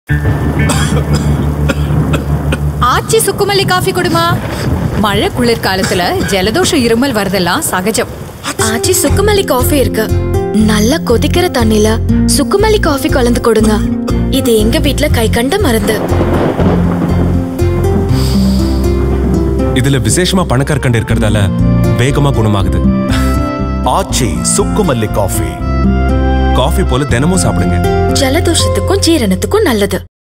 Aachi Chukkku Malli Coffee - ஜலதோஷத்திற்கும் ஜீரணத்திற்கும் ஆச்சி சுக்கு மல்லி காபி
Aachi Chukkku Malli Coffee - ஜலதோஷத்திற்கும் ஜீரணத்திற்கும் ஆச்சி சுக்கு மல்லி காபி - Good for Cold Relief and Digestion.

Very Simple to Prepare..